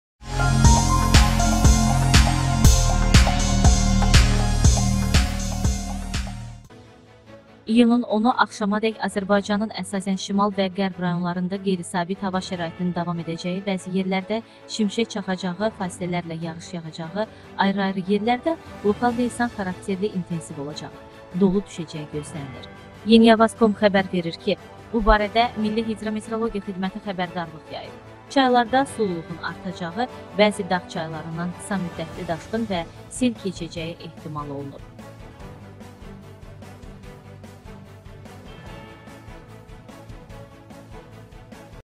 Rayonlarda hava qeyri-sabit keçəcək – XƏBƏRDARLIQ. Hava kəskin dəyişəcək - XƏBƏRDARLIQ
İyunun 10-u axşamadək Azərbaycanın əsasən şimal və qərb rayonlarında qeyri-sabit hava şəraitinin davam edəcəyi, bəzi yerlərdə şimşək çaxacağı, fasilələrlə yağış yağacağı, ayrı-ayrı yerlərdə lokal leysan xarakterli intensiv olacağı, dolu düşəcəyi gözlənilir.

Yeniavaz.com xəbər verir ki. bu barədə Milli Hidrometeorologiya Xidməti xəbərdarlıq yayıb.

Çaylarda sululuğun artacağı, bəzi dağ çaylarından qısamüddətli daşqın və sel keçəcəyi ehtimal olunur.